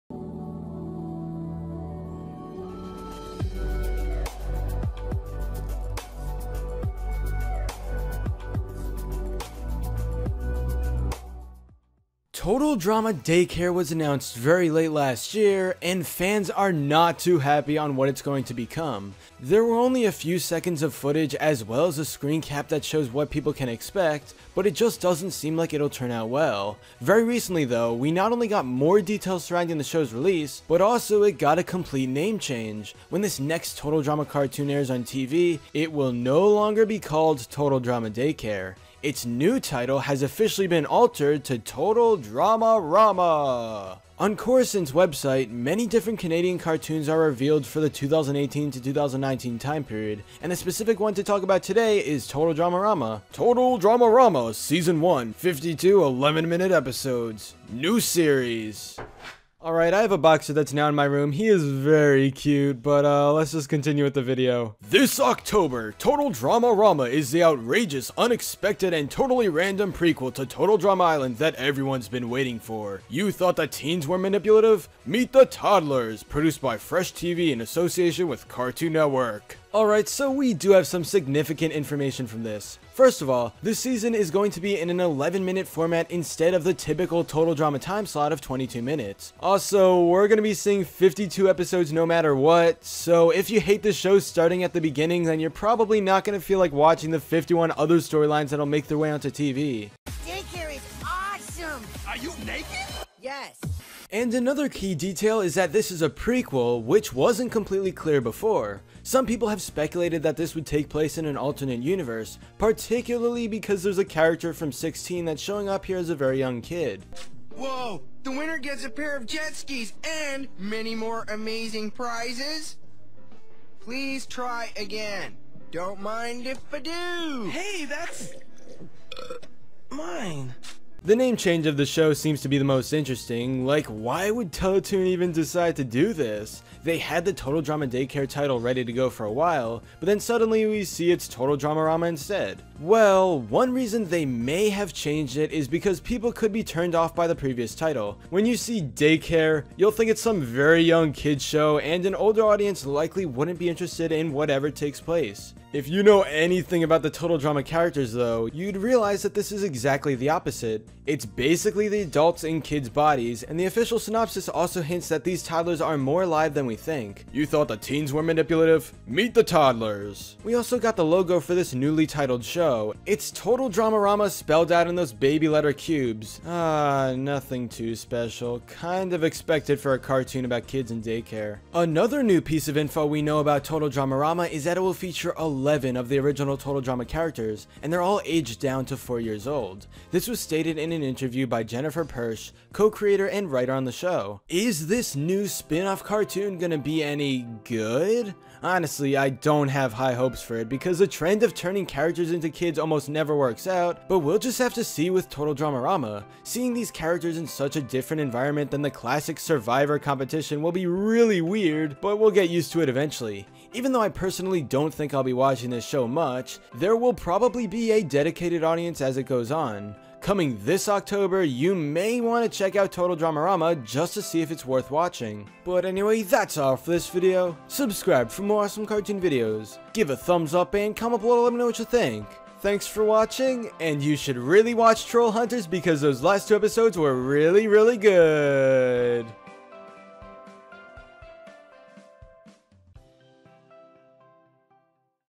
Total Drama Daycare's NEW NAME!
Total Drama Daycare is coming to Cartoon Network soon, but it won't be called Total Drama Daycare. It officially has a brand new title!